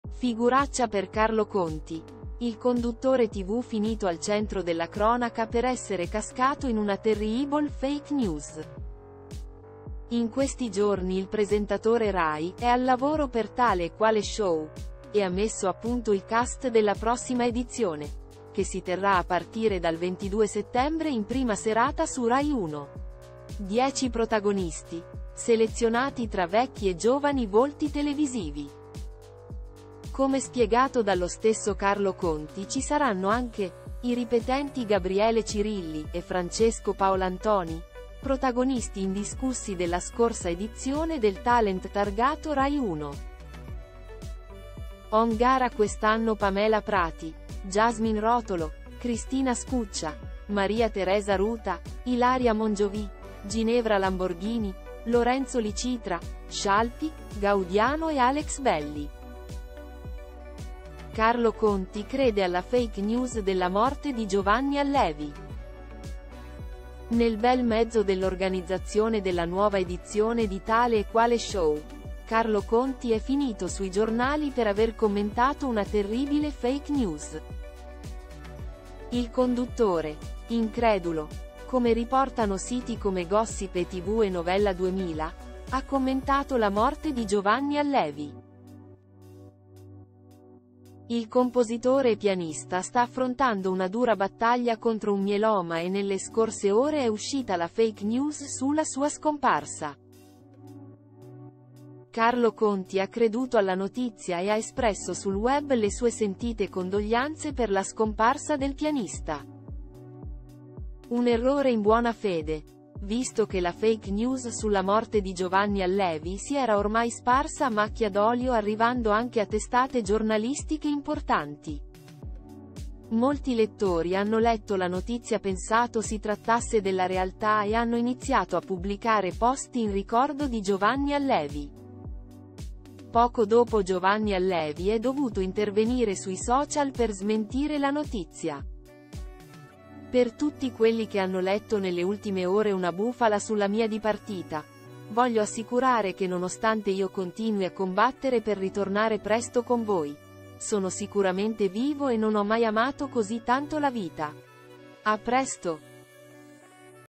“È mor.to, ci mancherà”. Poi arriva la verità: gaf.fe di Carlo Conti dopo la fal.sa notizia sul...
“È mor.to, ci mancherà”. Poi arriva la verità: gaf.fe di Carlo Conti dopo la fal.sa notizia sul famoso